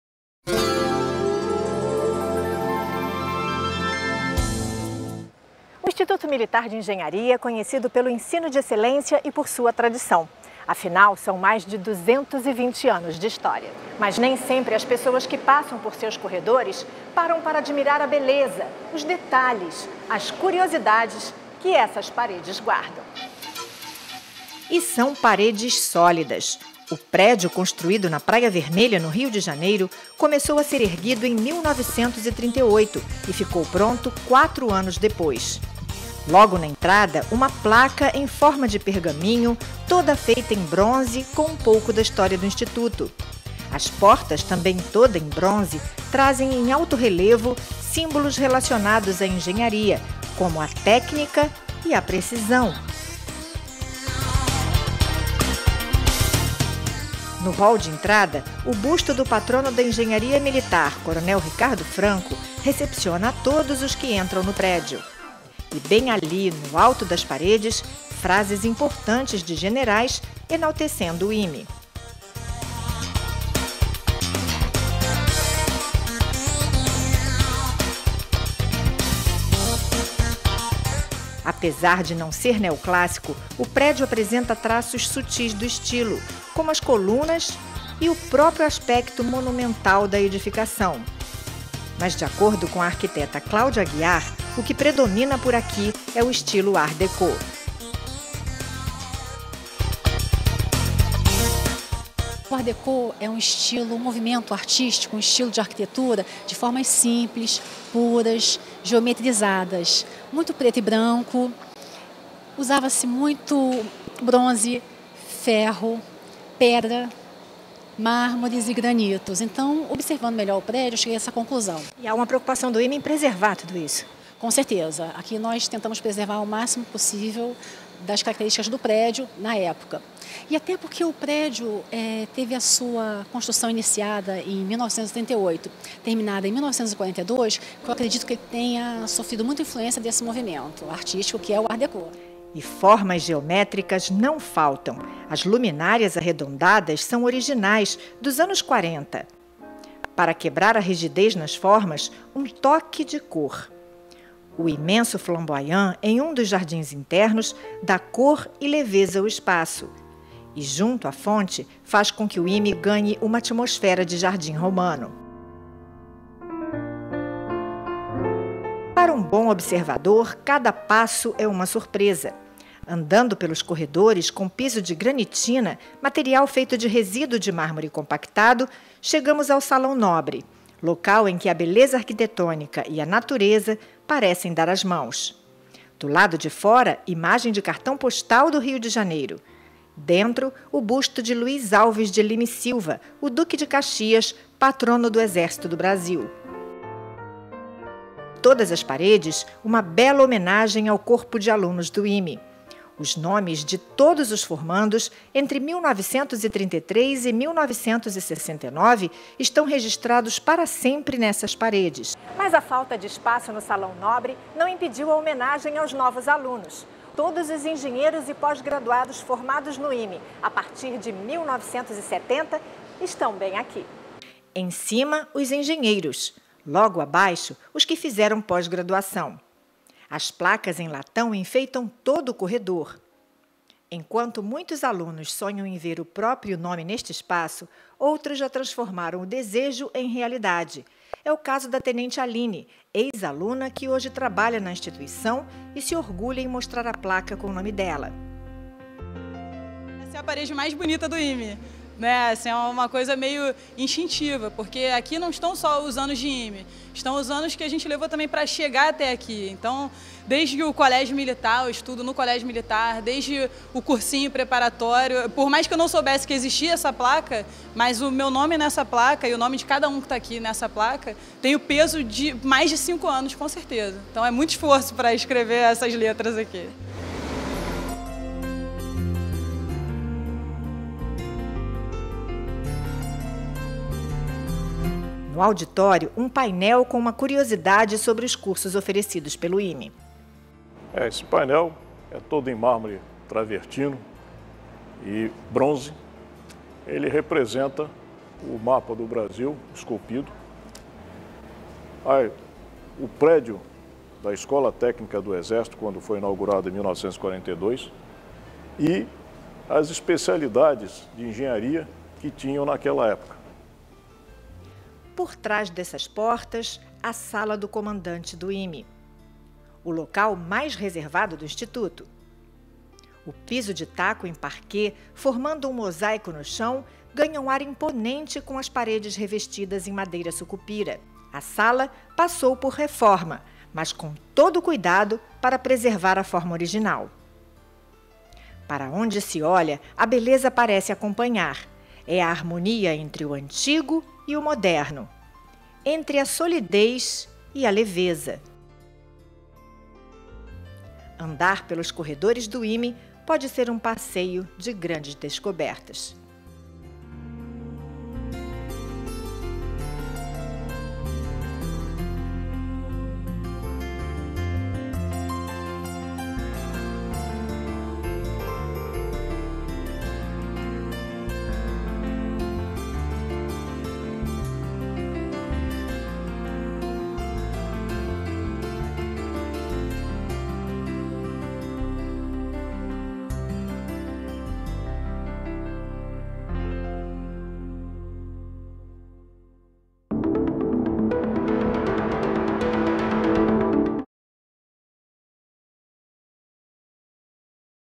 Arquitetura do IME: beleza singular
O prédio do Instituto Militar de Engenharia (IME) é uma construção que exibe uma beleza singular. Com influências Neoclássica e "Art Déco", sua arquitetura é um patrimônio, que deve ser preservado e valorizado.

Consagrado como a "Casa do Engenheiro Militar", esse espaço é marcado pelo bom gosto e pela harmonia das formas e dos ambientes.

Plenamente integrado ao local, o prédio do IME encontra-se incorporado à paisagem da Praia Vermelha, sob a vista do Pão de Açúcar.

"IME: o futuro é o nosso presente".